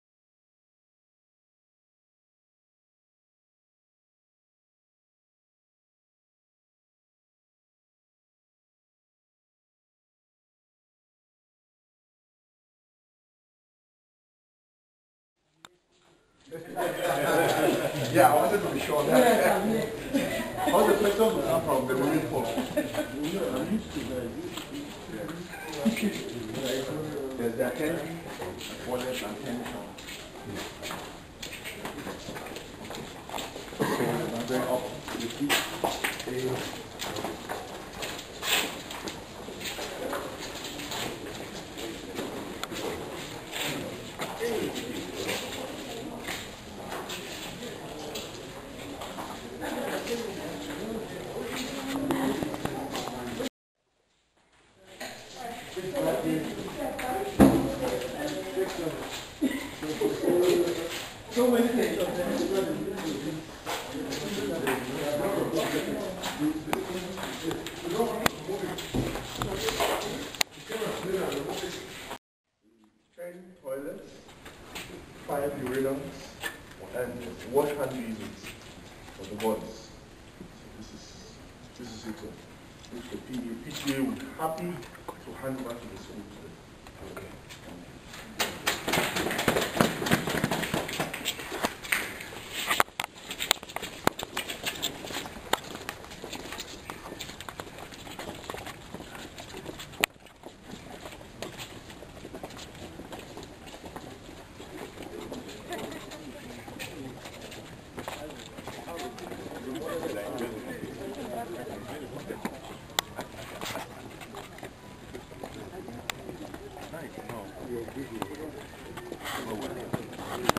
PSSRestrooms2.mp4
New water system restrooms for the boys dorm in PSS Mankon.  The new administration is proving to be getting things done.